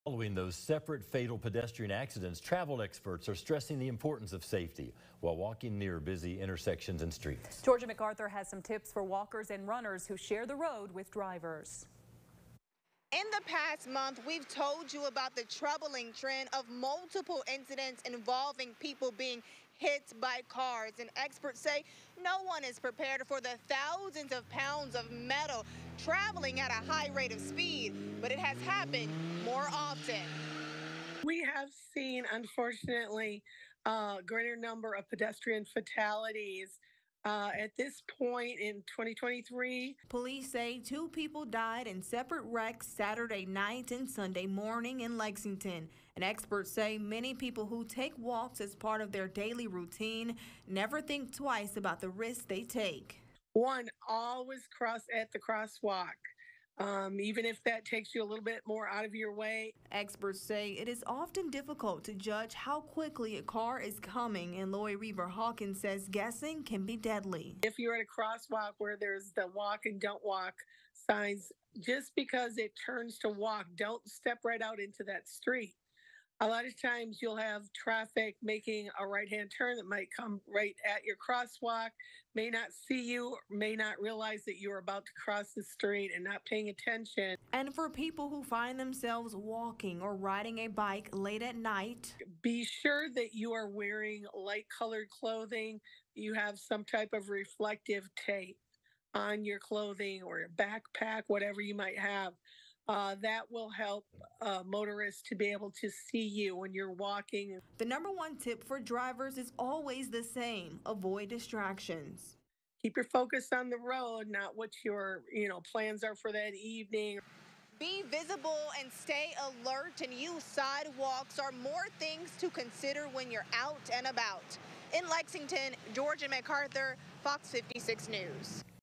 Tips to keep pedestrians safe in central Kentucky following string of accidents
Travel experts are stressing the importance of safety while walking near busy streets and intersections.